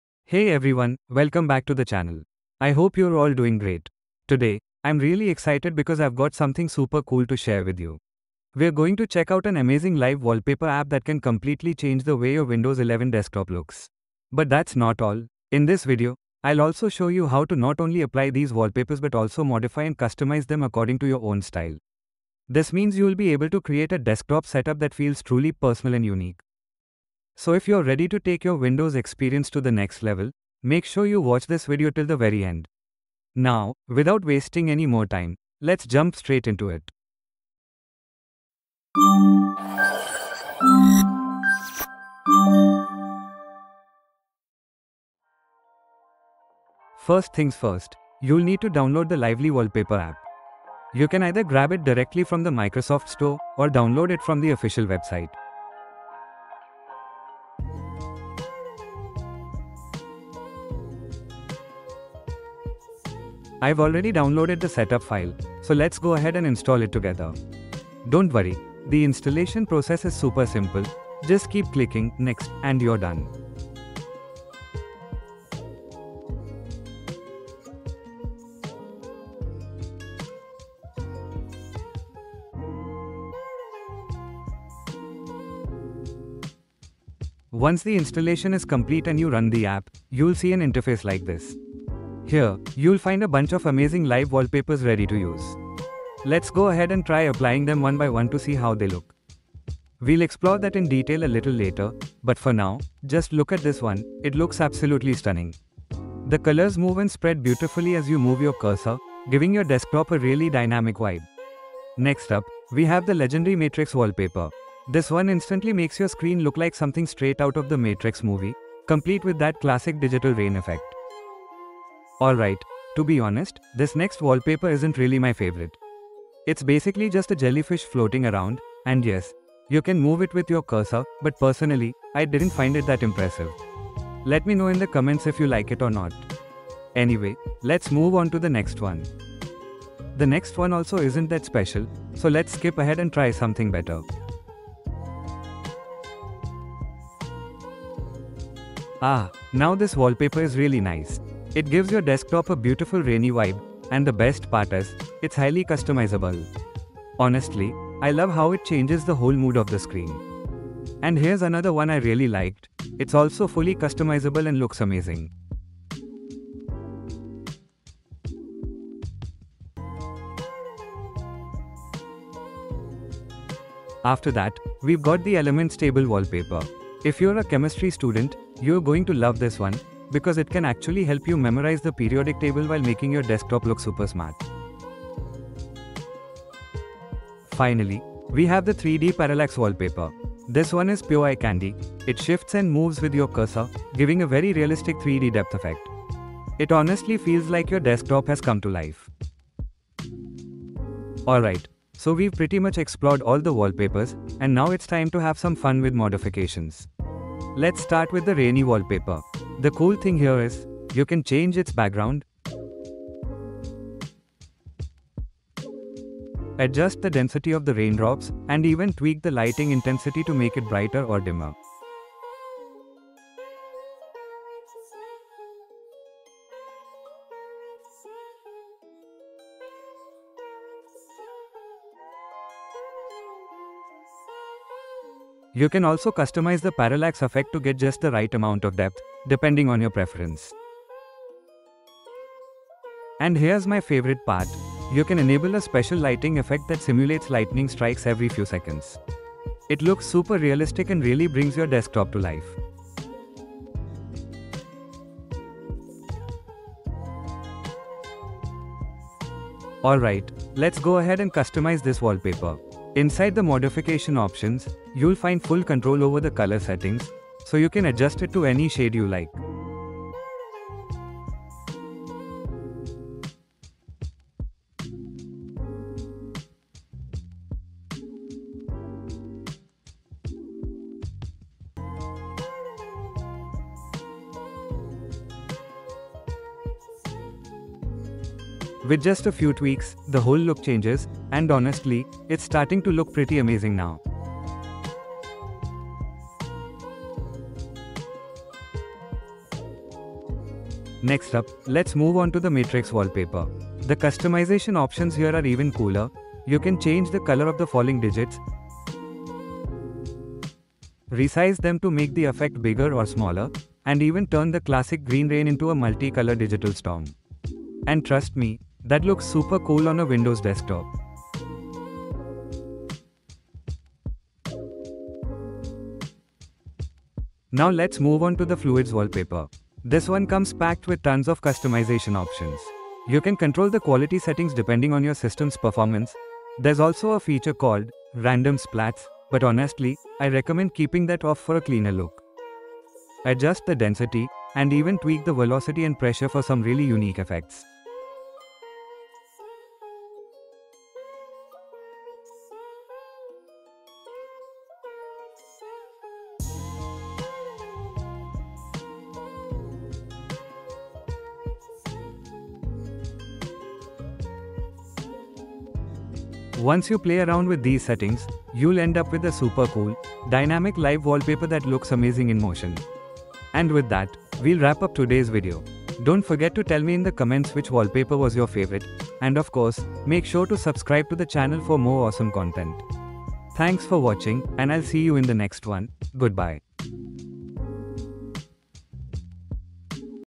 Best lively wallpapers and how to customize them!
Want to make your Windows desktop look alive? In this video, I’ll show you how to customize wallpapers in Lively Wallpaper and explore all the cool features it offers. From setting live wallpapers to adjusting quality, performance, and even adding your own videos or web pages as backgrounds—everything is explained step by step.

Whether you’re on Windows 10 or Windows 11, this tutorial will help you get the most out of the free and powerful Lively Wallpaper app.

✨ What you’ll learn in this video:

How to install and set up Lively Wallpaper

Explore built-in live wallpapers

Customize wallpaper settings (quality, performance, audio, etc.)

Import your own wallpapers (videos, GIFs, websites)

Tips to optimize Lively Wallpaper for smooth performance

🎥 If you love customizing your PC and making it unique, this guide is for you.